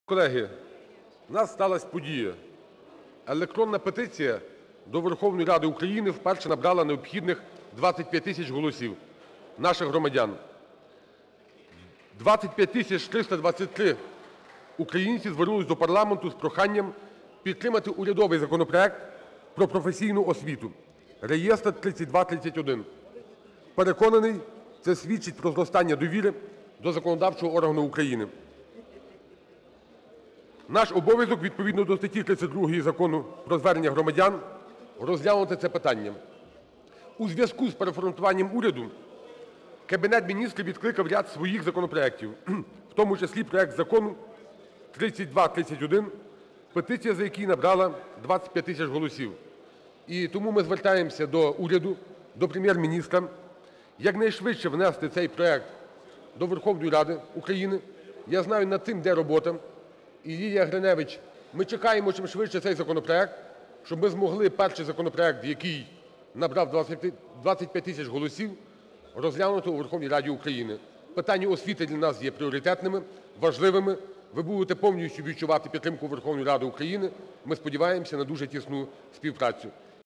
Верховна Рада розгляне першу електронну петицію до парламенту
Верховна Рада розгляне електронну петицію щодо закону "Про професійну освіту", яка вперше набрала необхідні 25 тисяч голосів українців.

Про це заявив Голова Верховної Ради, представник "Народного фронту" Андрій Парубій, відкриваючи засідання парламенту у п’ятницю.

"Електронна петиція до Верховної Ради України вперше набрала необхідних 25 тисяч голосів наших громадян. 25323 українці звернулися до парламенту з  проханням підтримати  урядовий законопроект  "Про професійну освіту" (№ 3231). Переконаний, це свідчить про зростання довіри до законодавчого органу України", - наголосив він.

"Наш обов'язок відповідно до статті 32 Закону "Про звернення громадян" - розглянути це питання", - підкреслив Андрій Парубій.

Проте спікер парламенту зауважив, що з огляду на переформатування уряду, Кабінет Міністрів відкликав ряд своїх законопроектів, в тому числі і проект закону № 3231, петиція щодо якого набрала понад 25 тисяч голосів. У зв’язку з цим Парубій звернувся  до Уряду та прем’єр-міністра з проханням якнайшвидше внести цей проект закону до Верховної Ради України.

"Ми чекаємо, аби ми змогли перший законопроект, який набрав понад 25 тисяч голосів, розглянути у Верховній Раді України", - сказав Голова парламенту, звертаючись до міністра освіти Лілії Гриневич.

"Питання освіти для нас є пріоритетними і важливими. Ви будете повністю відчувати підтримку з боку Верховної Ради України, і ми сподіваємося на дуже тісну співпрацю", - додав Голова парламенту.

Приєднуйся!